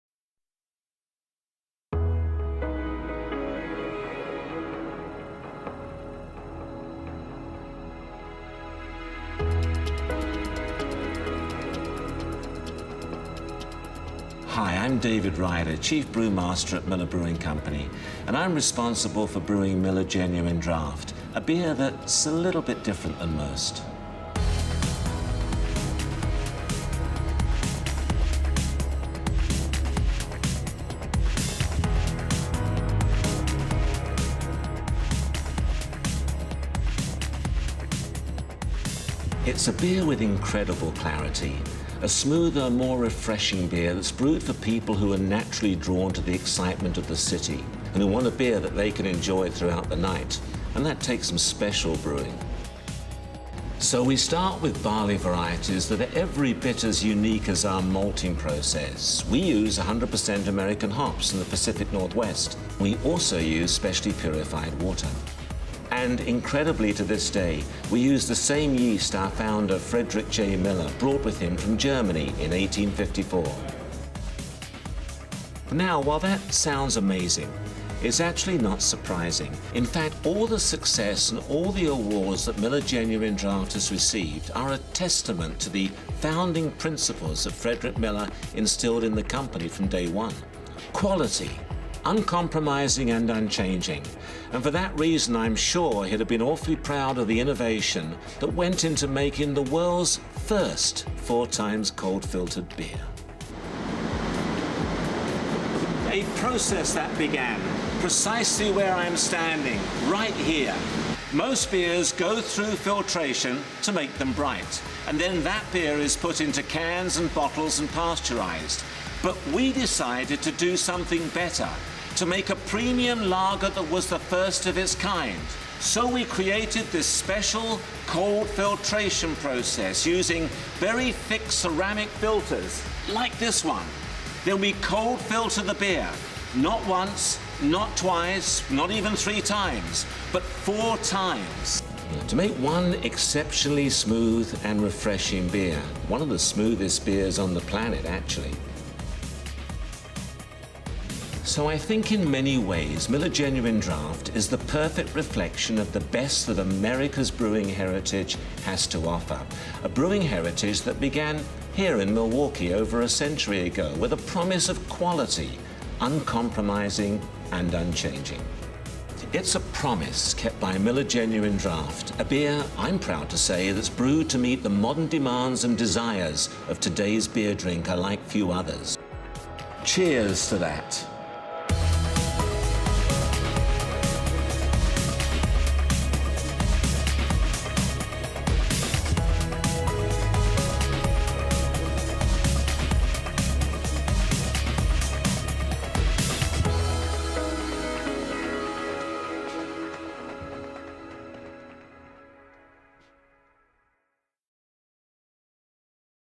The Brewing Process - How Miller Genuine Draft Is Made
Join Dr. David Ryder, Brewmaster at Miller Brewing Company, as he describes the process that goes into brewing Miller Genuine Draft. Dr. David Ryder is the Vice President, Brewing Research & Quality Assurance at Miller Genuine Draft.

Please drink responsibly.